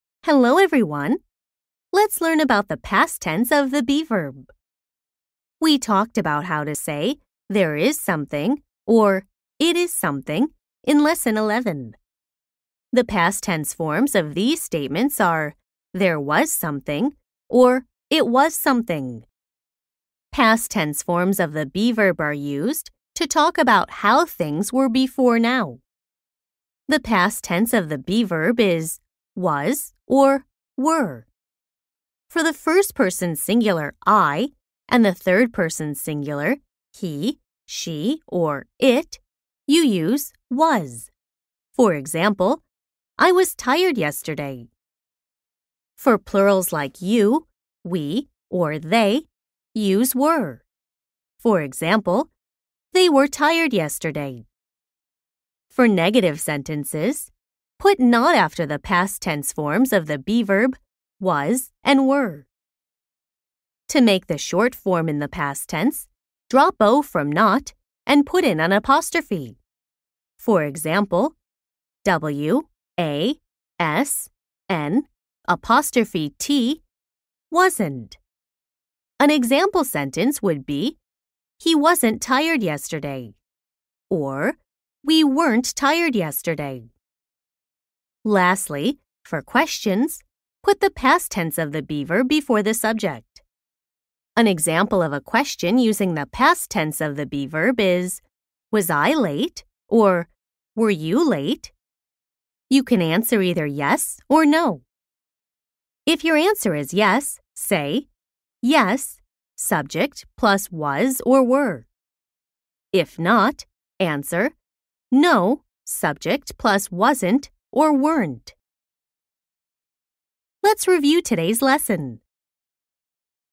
[ Basic English Grammar Lesson ] Lesson 12 : Be verbs (past)
Let’s learn about past tense of ‘be-verb’
To say ‘there was something’, or ‘it was something’, you use the past tense of be verb.
Be verb past tense includes ‘was’ and ‘were’.
For first person singular ‘I’, and third person singular ‘He, she’, you use ‘was’. For example, “I was tired yesterday.”
For plurals like ‘You, we, they’, use ‘were’ to say ‘They were tired yesterday”.
For negative sentence, put ‘not’ after the past tense of be verb ‘was and were’.
To make it short, drop ‘o’ from ‘not’ and put an ‘apostrophe’ like “He wasn’t tired yesterday.”  “We weren’t tired yesterday.”
Lastly, for question sentence, put the past tense of be verb before the subject. It looks like“Was I late?” “Were you late?”
You can either answer ‘Yes’ or ‘No’, and if you agree with the question, say “Yes, subject plus was or were’ or if not, ‘No, subject + wasn’t or weren’t’
“Let’s review today’s lesson”